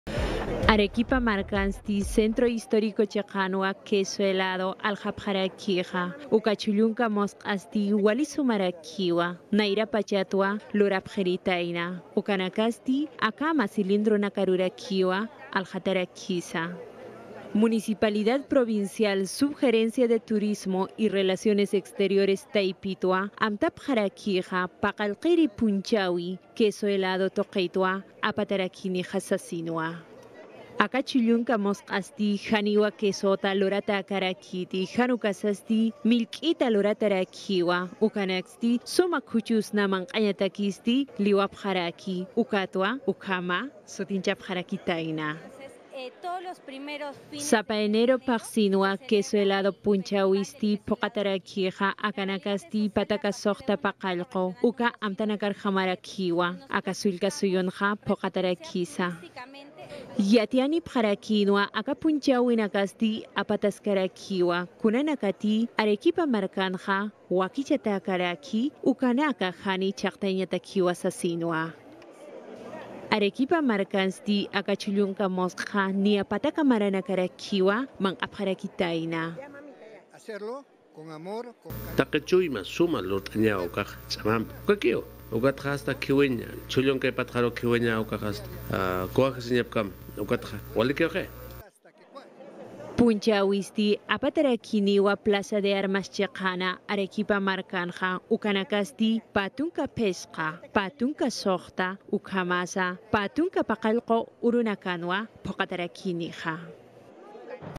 "El Queso helado" sutini phunchawiwa Arequipa tuqina wakichasi
"El Queso helado" sutini phunchawiwa Arequipa tuqina wakichasi. Uka yatiyawi khananchapxañani